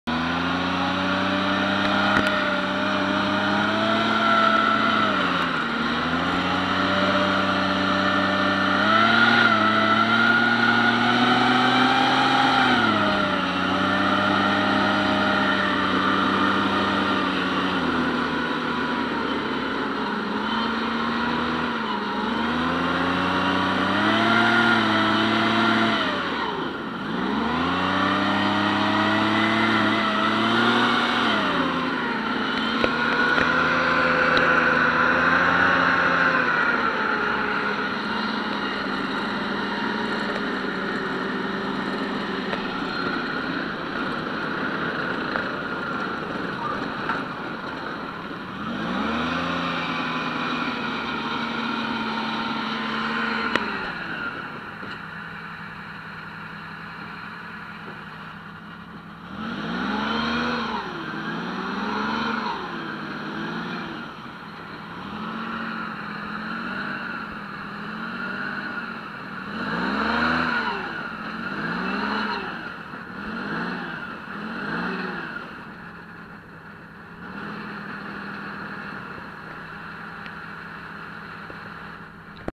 Out for a ride in a Canadian Army armoured recovery vehicle at the Canadian War Museum
Out for a ride in a Canadian Army armoured recovery vehicle during Tank Day at the Canadian War Museum in Ottawa, 27 August 2016.